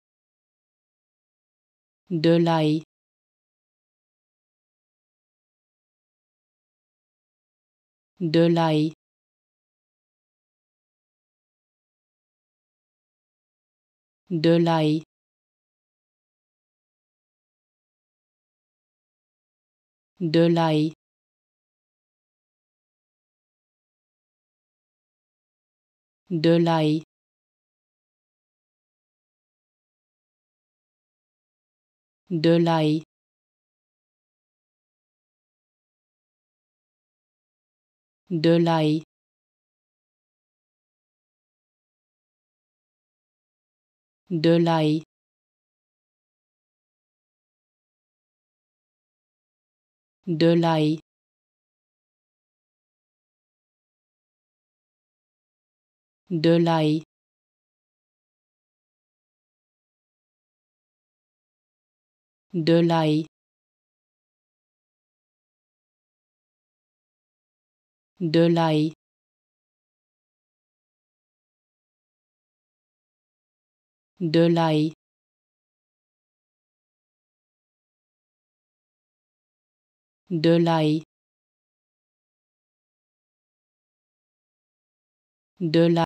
How to Pronounce ''Delaille'' Correctly in French
Learn how to say and properly pronounce ''Delaille'' in French with this free pronunciation tutorial.